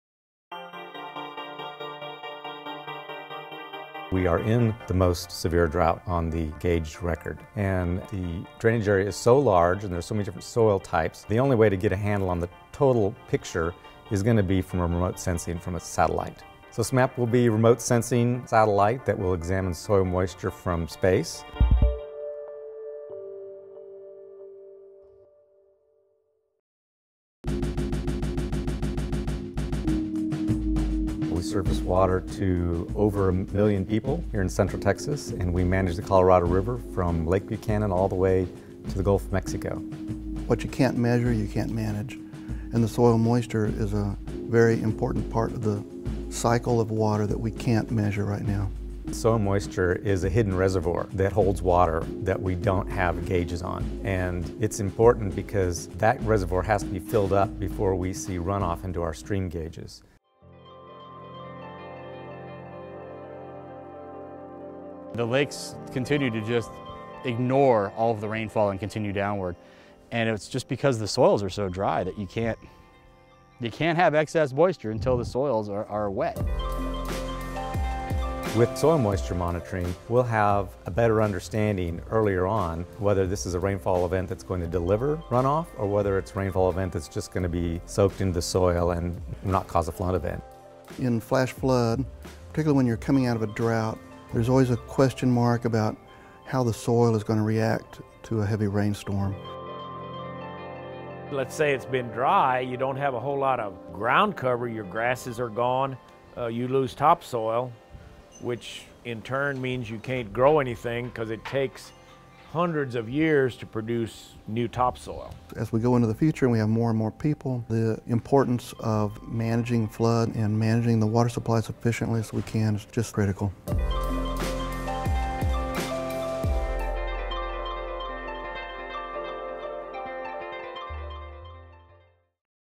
NASA’s SMAP Satellite Monitors Texas Drought
NASA's Soil Moisture Active Passive satellite gives Texas water agencies critical information for managing the lone star state's limited water.